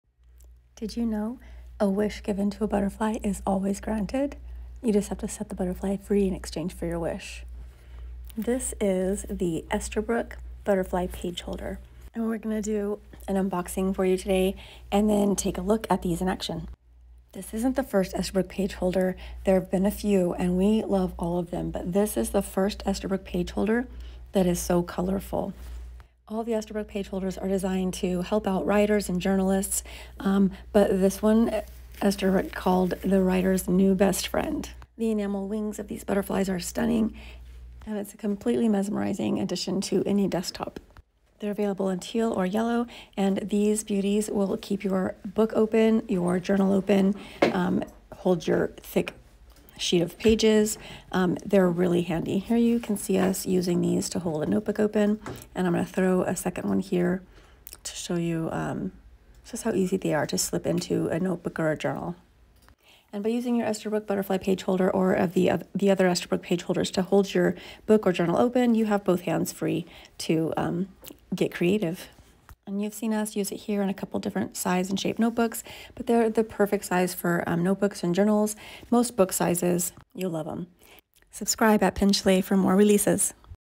Upgrade Your Note-Taking: Esterbrook Butterfly Holder
Watch the Esterbrook Butterfly Page Holder unboxing video today for a closer look at these stunning new page holders that we know you are dying to have!

Highlights about the Esterbrook Butterfly Page Holder:
• Designed to be the writer's new best friend
• Available in Teal or Yellow
• Keeps your journal or book open
• The perfect size for notebooks, journals, and most book sizes

Hope you enjoyed this Esterbrook Butterfly Page Holder unboxing video. Shop for your own Esterbrook Butterfly Page Holder at PenChalet.com.